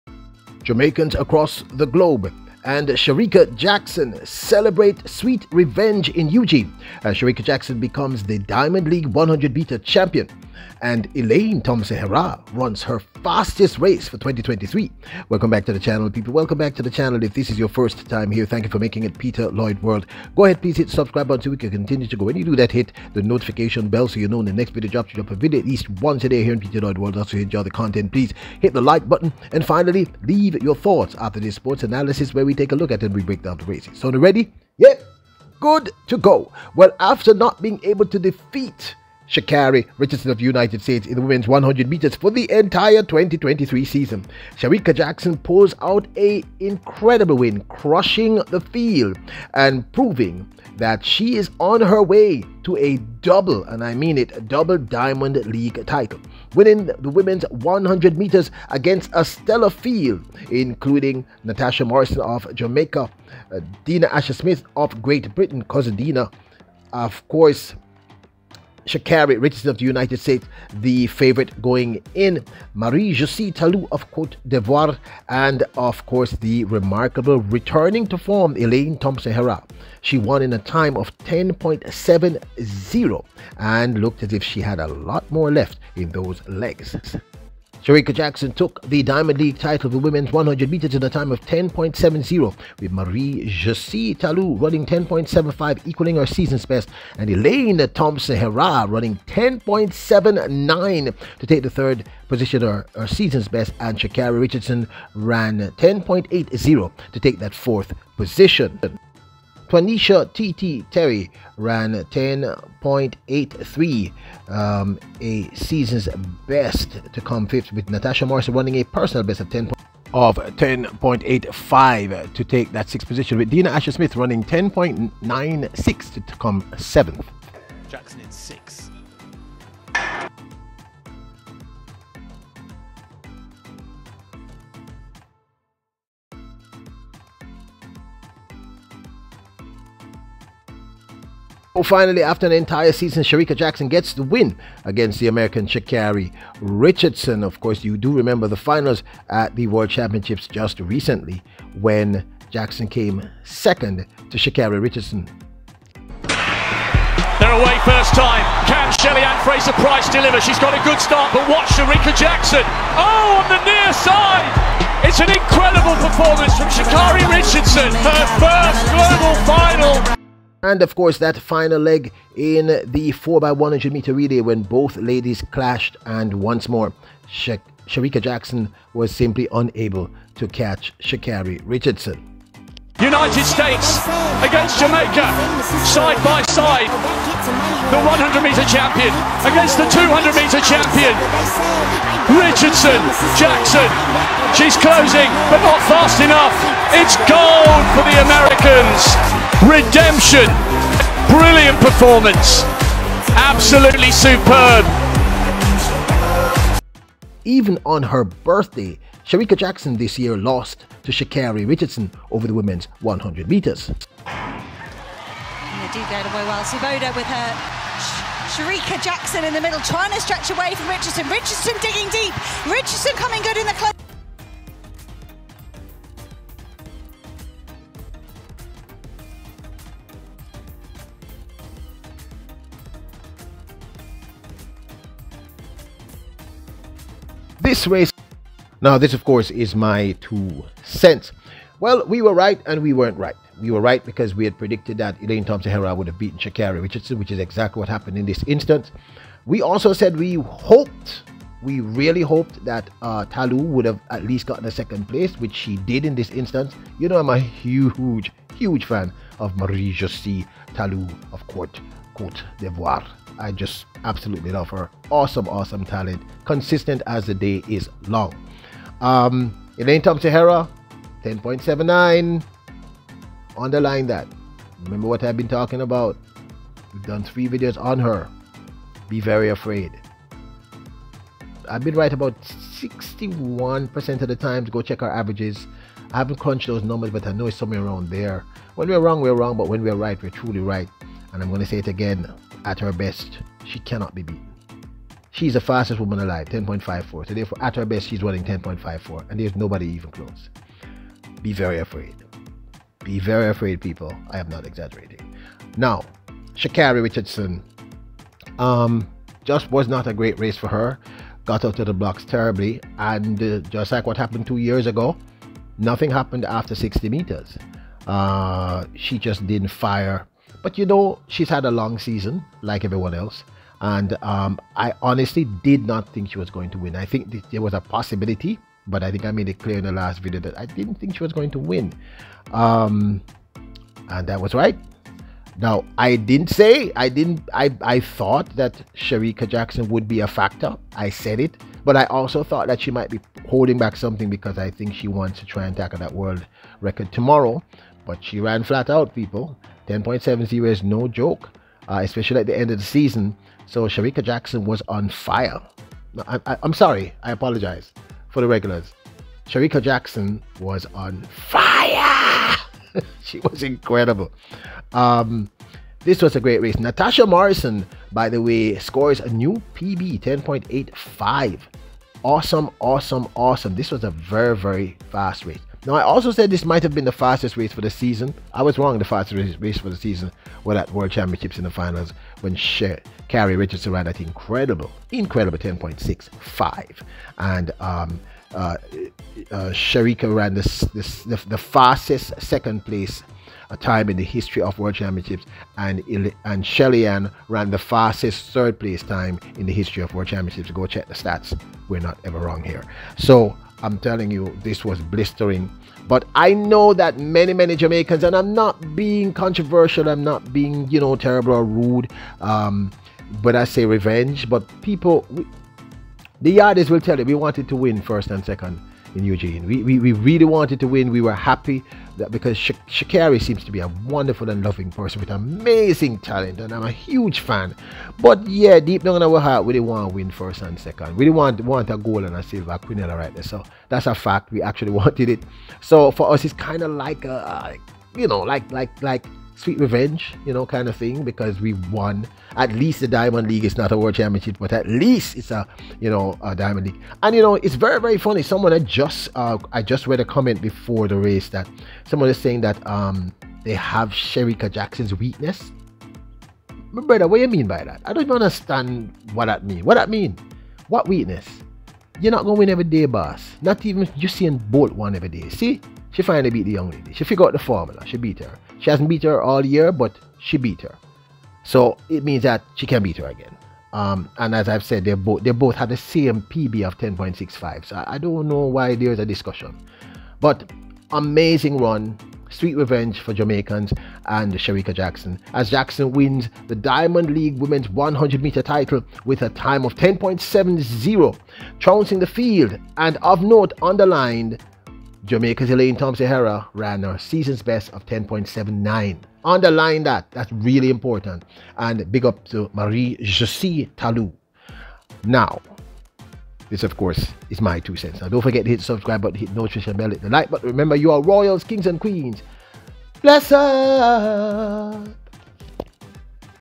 JAMAICANS & SHERICKA JACKSON CELEBRATE 100M REVENGE IN EUGENE! JACKSON IS DIAMOND LEAGUE CHAMP !!!
JAMAICANS & SHERICKA JACKSON CELEBRATE 100M REVENGE IN EUGENE! SHERICKA JACKSON IS DIAMOND LEAGUE 100M CHAMPION FOR 2023 !!!